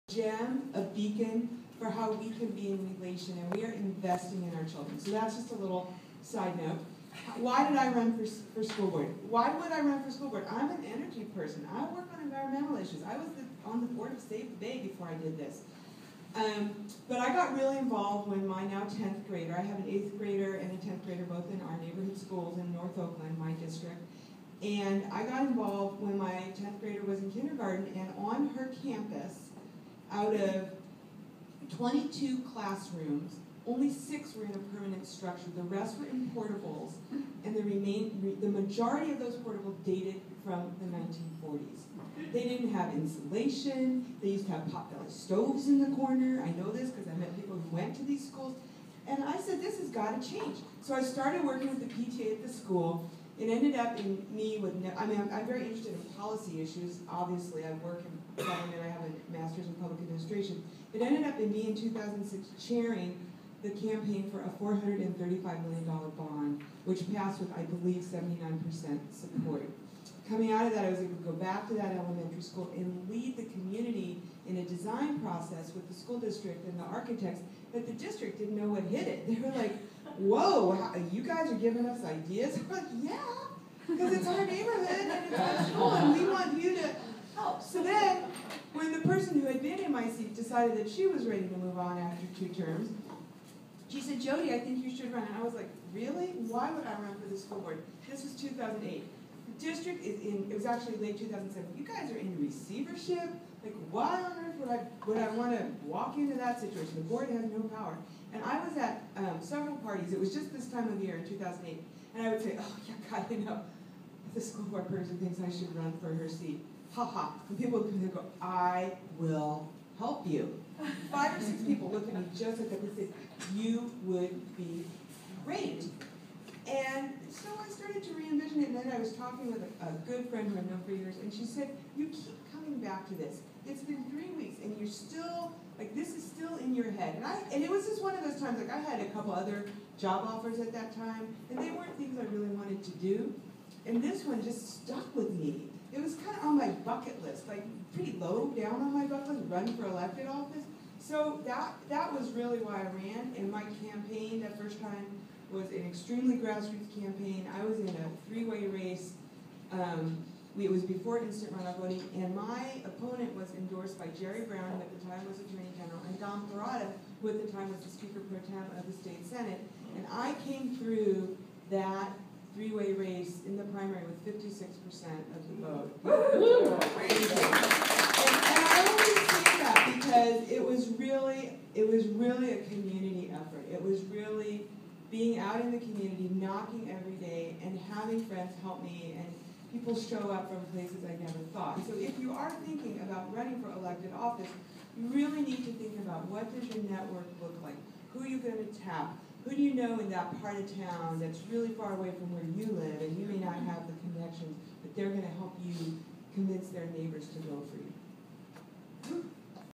Women's Empowerment Day in Oakland- Empowering Your Leadership Panel (Part III)
This one-day conference gave women from throughout the state the skills and tools to be empowered leaders. Featured Speakers for this morning panel on Empowering Your Leadership - Perspectives from Women Leaders included Courtney Ruby, Member, California Women Lead State Board of Directors, Jumoke Hinton Hodge, Governing Board Member, Oakland Unified School District 3, Jody London, Governing Board Member, Oakland Unified School District 1, Roseann Torres, Governing Board Member, Oakland Unified School District 5 and Anne Campbell Washington, Governing Board Member, Oakland Unified School District 4